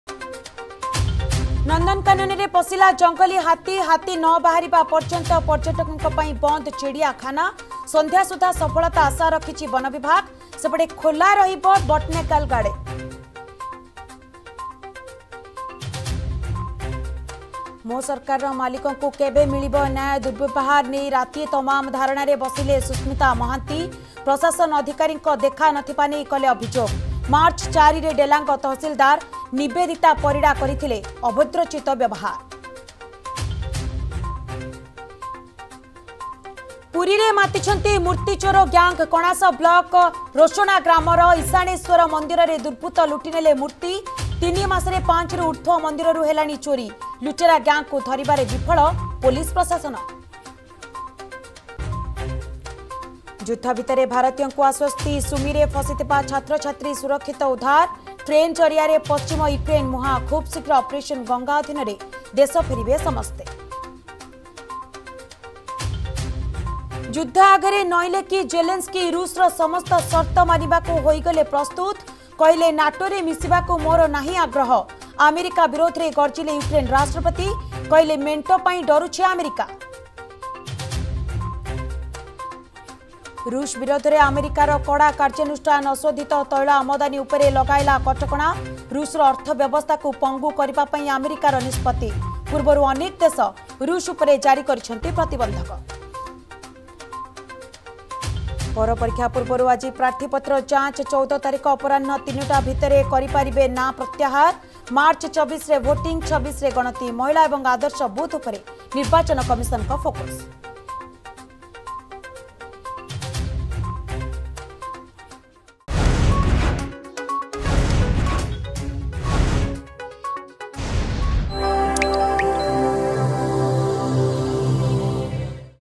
Headlines 10 am | 09 Mar 2022 | Argus News
+ନନ୍ଦନକାନନରେ ପଶିଲା ଜଙ୍ଗଲୀ ହାତୀ...
+ମୋ ସରକାରର ମାଲିକକୁ କେବେ ମିଳିବ ନ୍ୟାୟ…
+ପୁରୀରେ ମାତିଛନ୍ତି ମୂର୍ତ୍ତି ଚୋର ଗ୍ୟାଙ୍ଗ….
+ଯୁଦ୍ଧ ଭିତରେ ଭାରତୀୟଙ୍କୁ ଆଶ୍ବସ୍ତି...
+ଯୁଦ୍ଧ ଆଗରେ ନଇଁଲେ କି ଜେଲେନସ୍କି…
+ଋଷ ବିରୋଧରେ ଆମେରିକାର କଡ଼ା କାର୍ଯ୍ୟାନୁଷ୍ଠାନ…
+ପୌର ପରୀକ୍ଷା ପୂର୍ବରୁ ଆଜି ପ୍ରାର୍ଥୀପତ୍ର ଯାଞ୍ଚ...